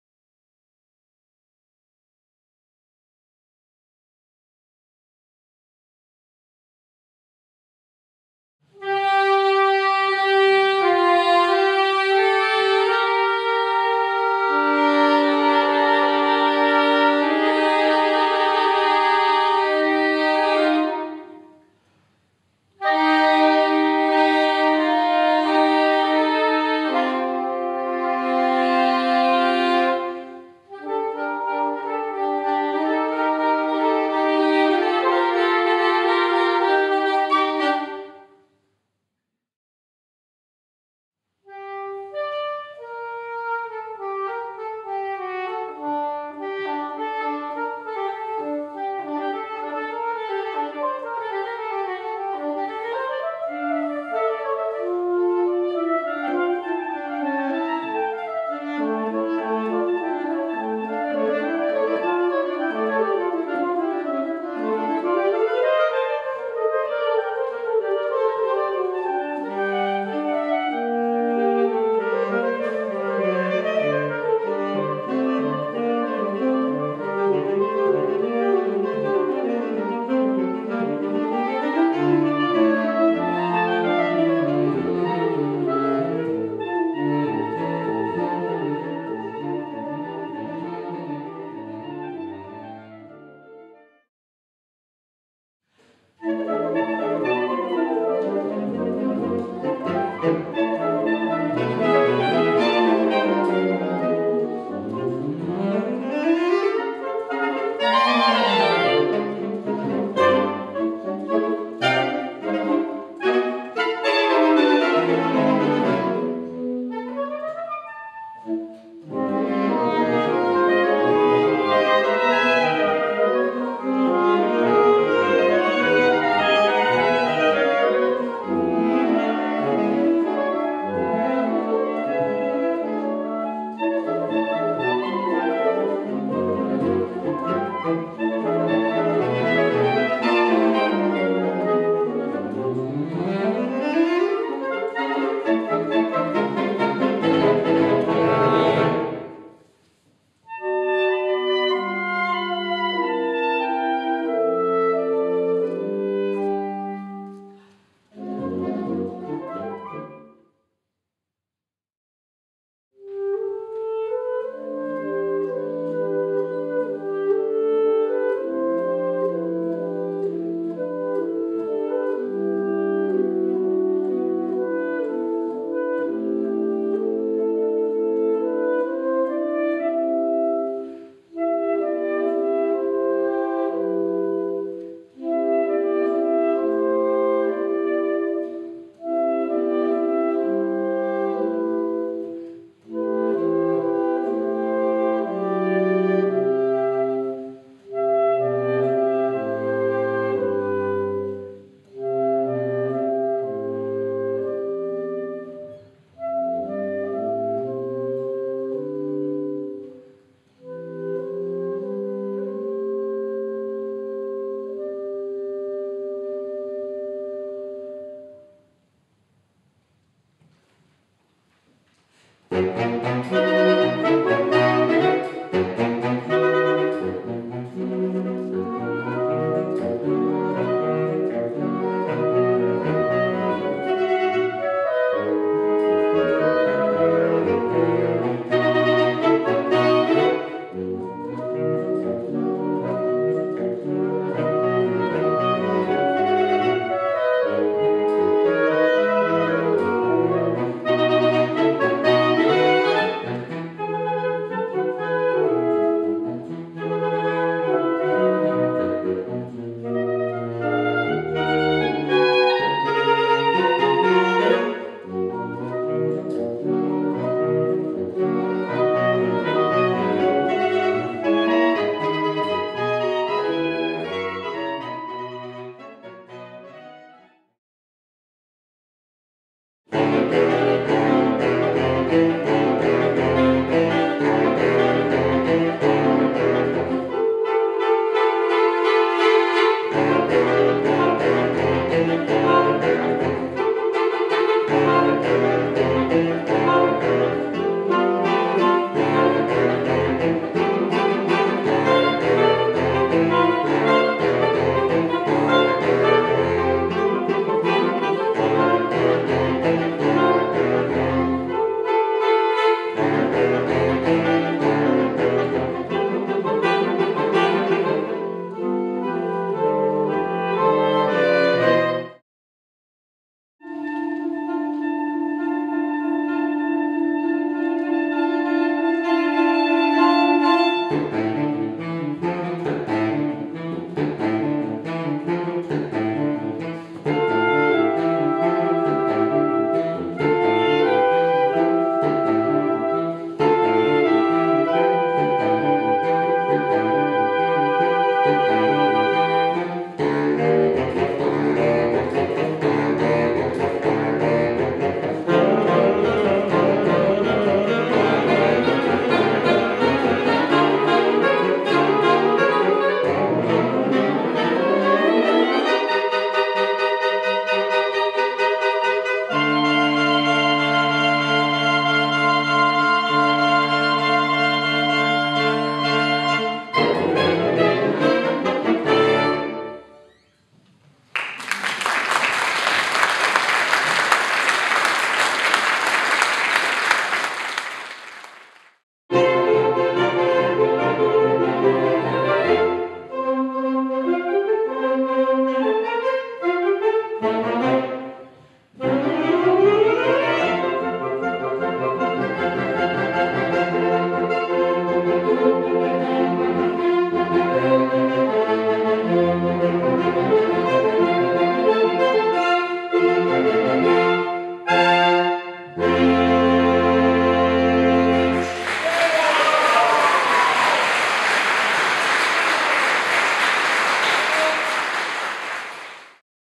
Borealis Saxophone Quartet - Recital Sampler (Recorded live at Sittingbourne Music Society)
A compilation of clips taken from the Borealis Saxophone Quartet's recital at Sittingbourne Music Society on 9/11/2018.

Alastair Penman - Soprano Saxophone
Gillian Blair - Alto Saxophone
Victoria Puttock - Tenor Saxophone
Tom Gimson - Baritone Saxophone